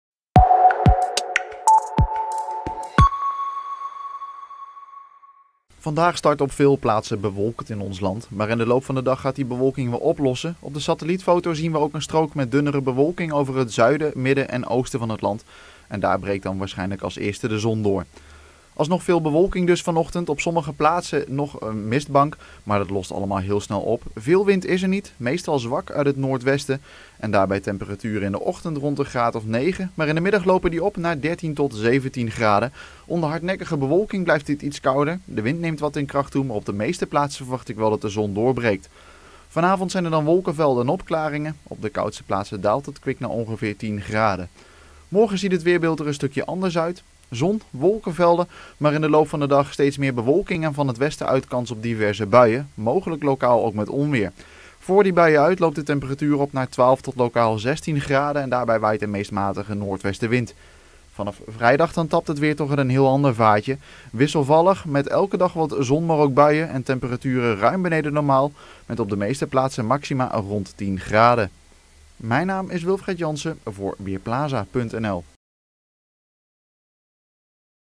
Weerbericht woensdagochtend: geleidelijk meer zon
Vooral in het zuiden kunnen eerst enkele mistbanken voorkomen. Verder is het vanochtend overwegend bewolkt, vanmiddag komt de zon op steeds meer plaatsen tevoorschijn. Het blijft droog, er staat maar weinig wind en het wordt 13 tot 18 graden. Donderdag trekt de wind wat aan en hebben we een afwisseling van zon, stapelwolken en enkele buien. Het wordt dan ongeveer 14 graden. Vrijdag en zaterdag houden de buien aan. Bij een verder toenemende noordwestenwind komt de temperatuur dan niet veel hoger meer dan 10 graden.